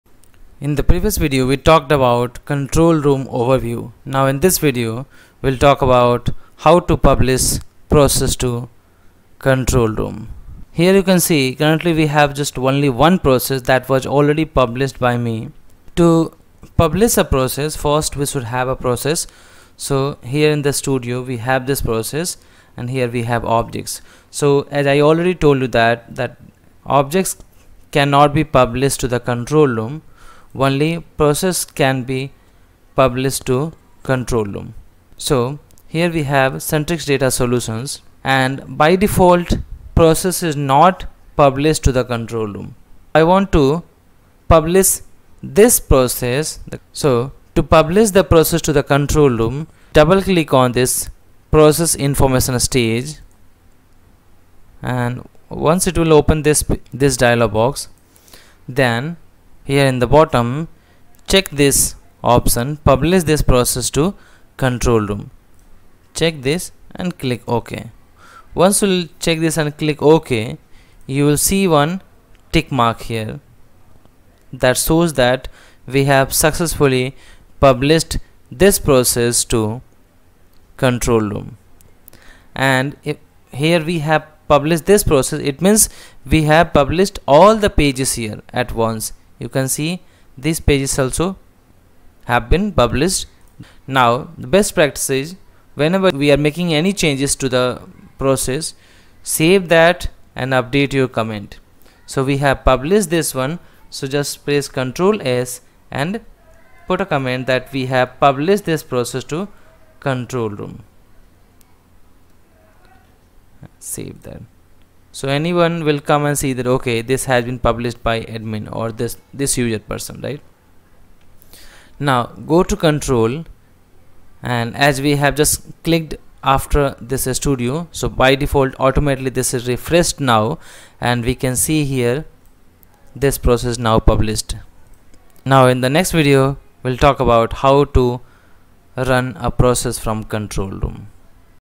Publish Process to Control Room in Blue Prism | Session 49 | BluePrism RPA
👉 Video Agenda - Publish Process to Control Room in Blue Prism
How to publish project to Control Room in Blue Prism?
Where to Find Published Process in Blue Prism?